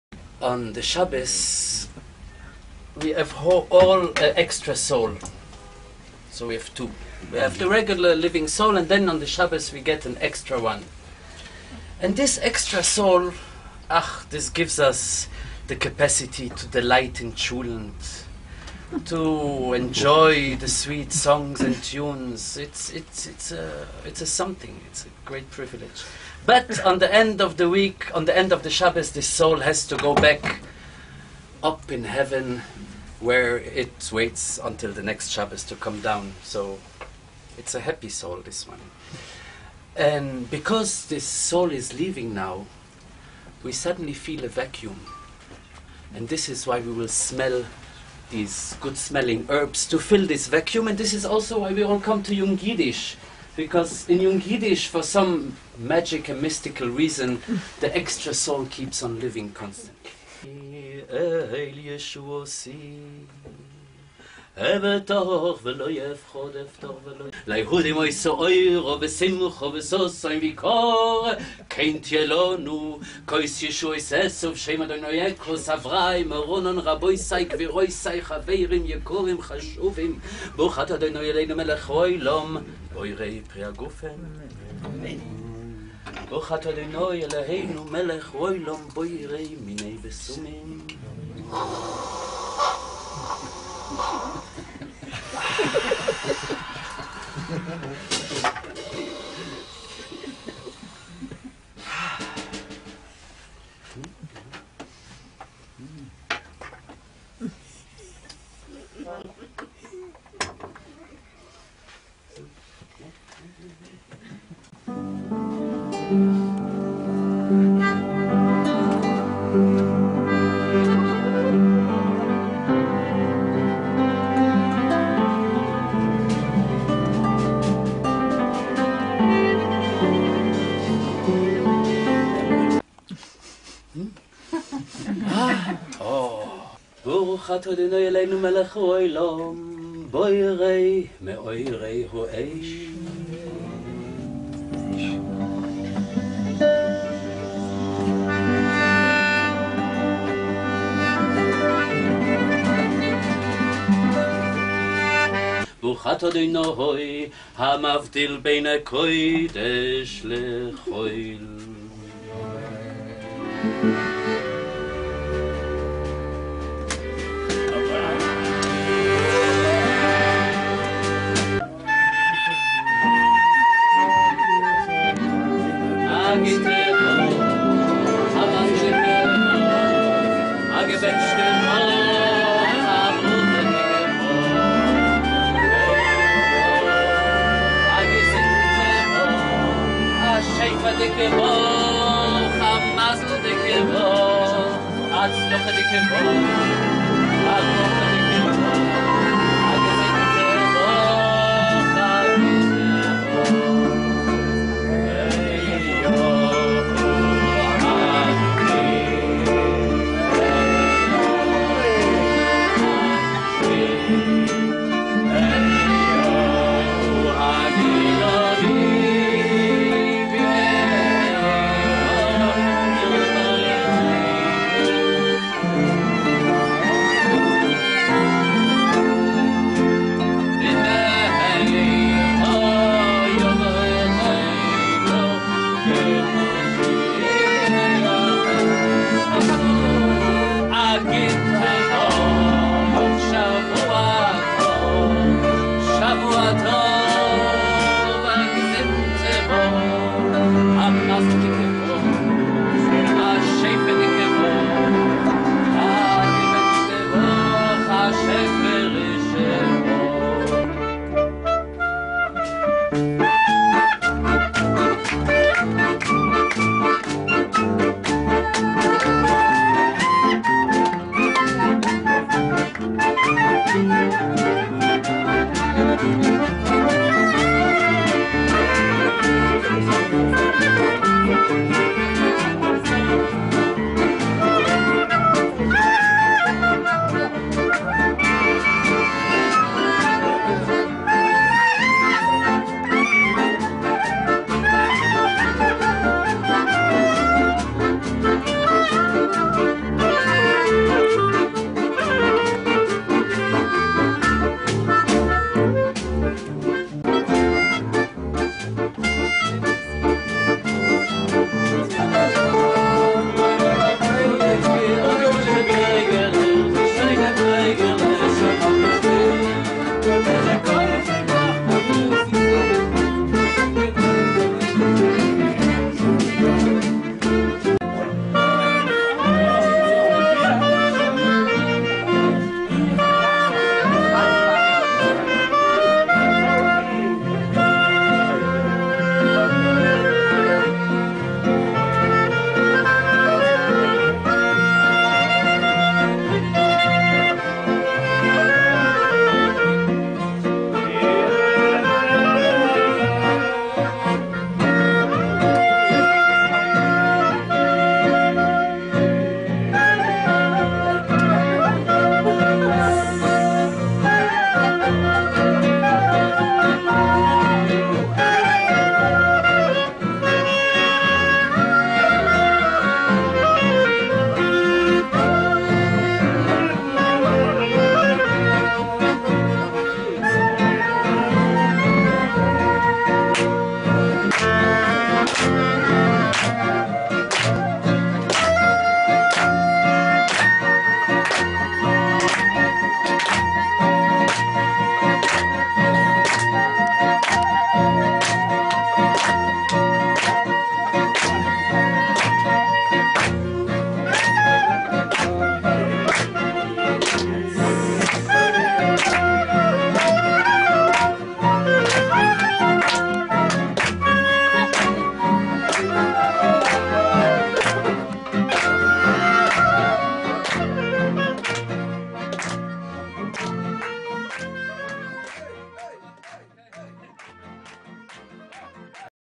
Jerusalem Klezmer Club
Saturday night
 Yung Yiddish Jerusalem
 Jerusalem Klezmer Association meetings
 "Havdalah" by Mendy Cahan
 And then, klezmer musicians and the audience sang
 Lulu Ducky Photo Contributor: Lior Grossman accordion
 Avrum Burstein clarinet and management
 Nachman Zucker piano and trombone
 Jonathan Mgidis Drums and dance
 Menachem Burstein flute
 Offer Schneider Double Bass
 T. Gafni melodica
 Dov Silberman violin
 Levi Yitzhak Maurer guitar
 Mendy Cahan singer and actor
מוצאי שבת
יונג יידיש ירושלים
מפגשי אגודת הכליזמרים ירושלים
"טקס הבדלה"  מאת מנדי כהאן
ואחר כך, הכליזמרים מנגנים והקהל שר
צילום לולו דאקי משתתפים: ליאור גרוסמן אקורדיון
אברום בורשטיין קלרינט וניהול
נחצן צוקר פסנתר וטרומבון
יונתן מגידיש תופים וריקוד
מנחם בורשטיין חליל צד
עופר שניידר קונטרבס
. גפני מלודיקה
דב זילברמן כינור
לוי יצחק מאורר גיטרה
מנדי כהאן זמר  ושחקן